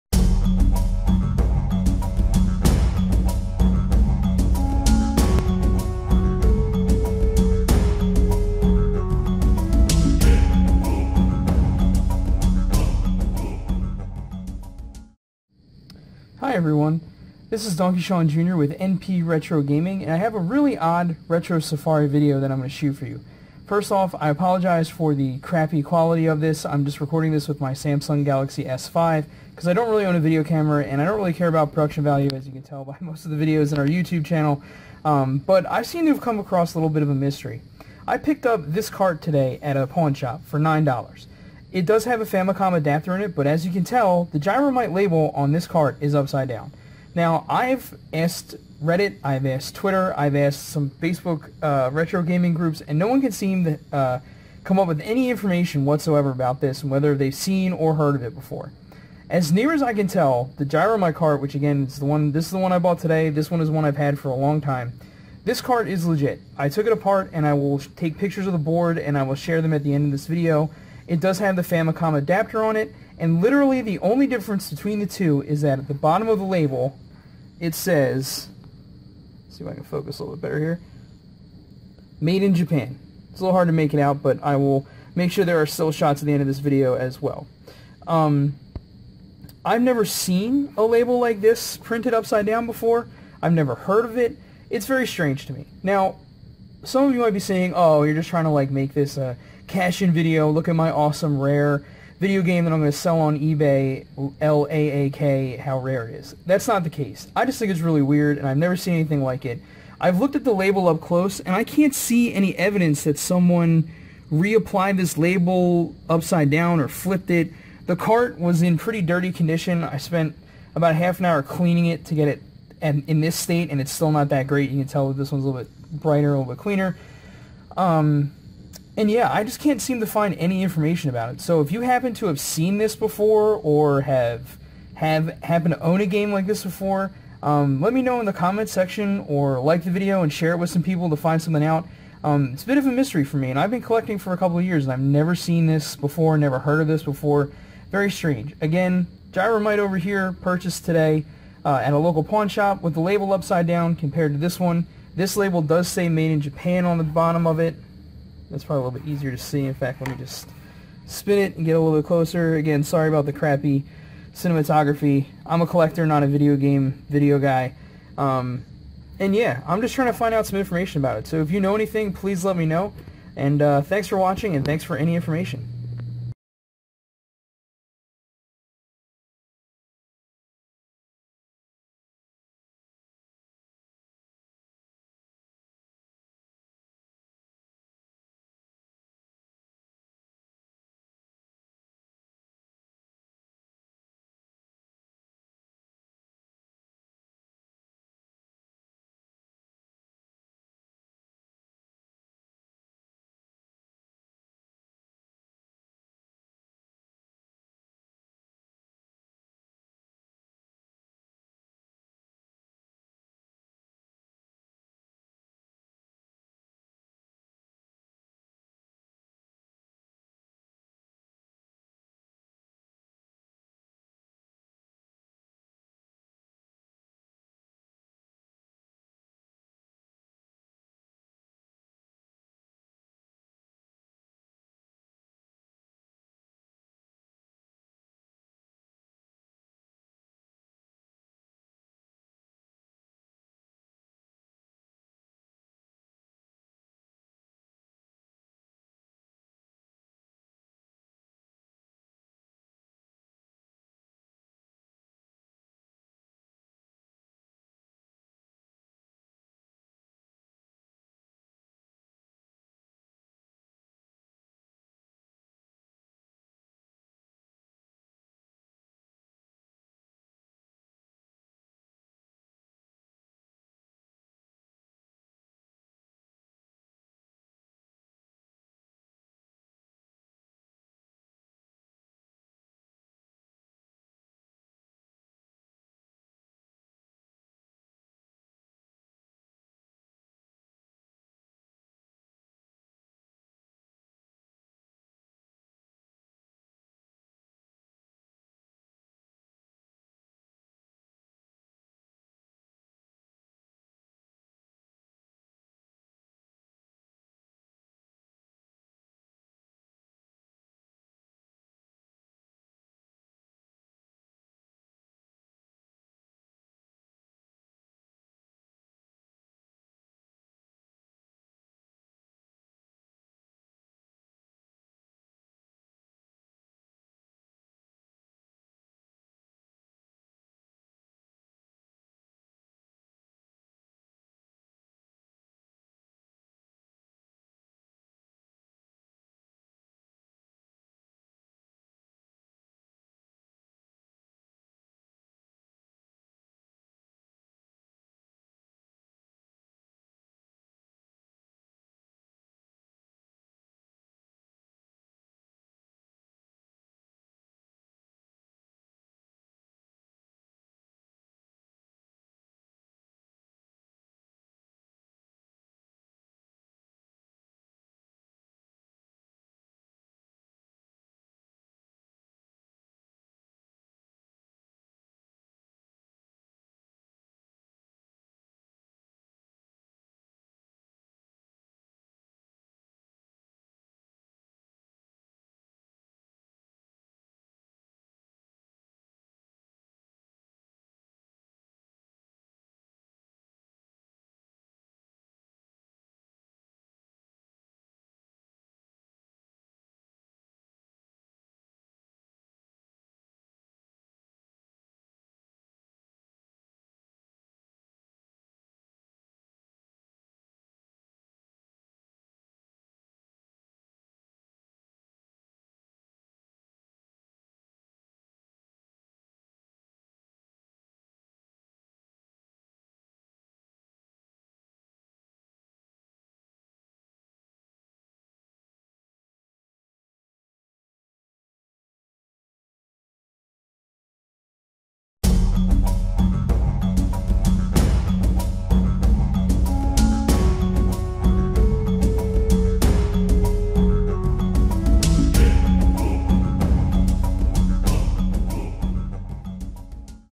Retro Safari - Upside down Gyromite!?!
So today at a Pawn Shop near where I live I found this weird Gyromite cart. The label is upside down. I have never seen nor heard of this before. If anyone has any info I would really appreciate it